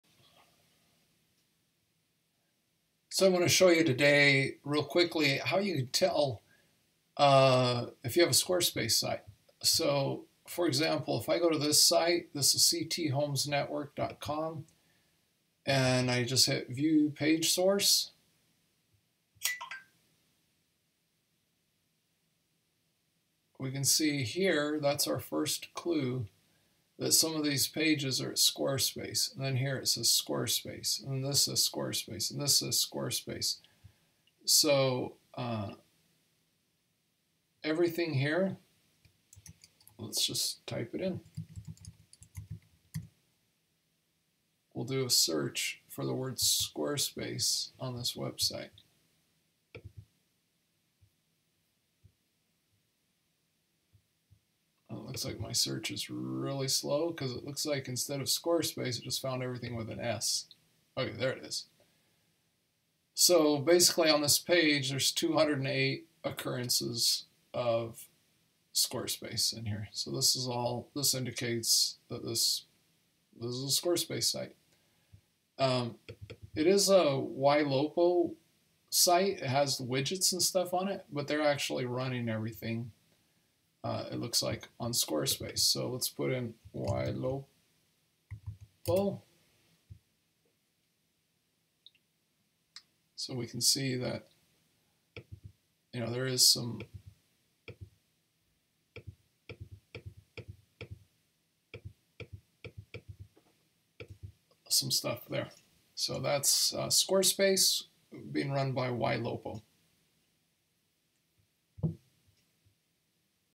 How to tell if your YLOPO site is using SquareSpace
It turns out that it is built on SquareSpace.

So basically our IDX Broker clients who use SquareSpace like the ones can be built to integrate IDX Broker and your SquareSpace website.